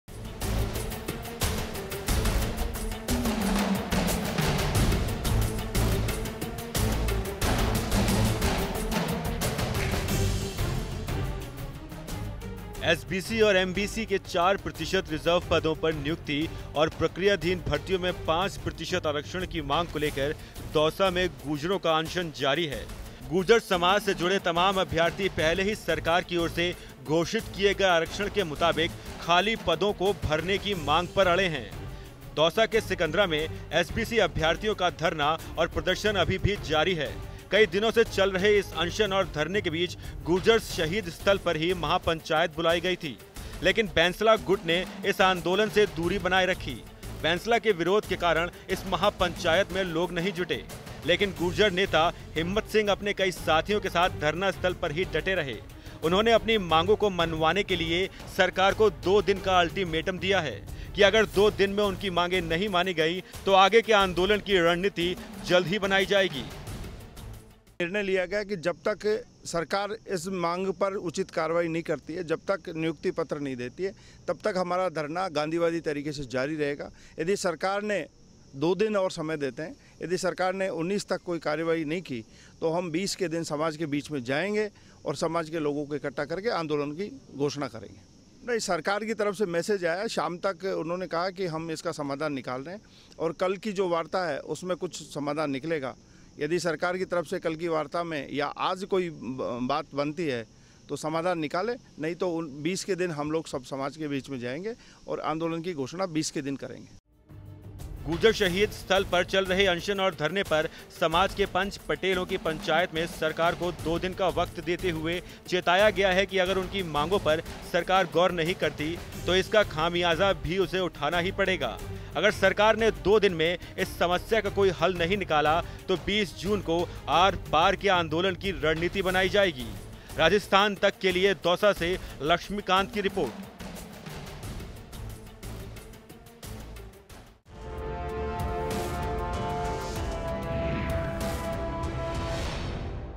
दौसा में गुर्जर आंदोलन !
दौसा में एक बार फिर सुलगी गुर्जर आरक्षण की आग, बैंसला विरोधी गुट ने संभाली कमान !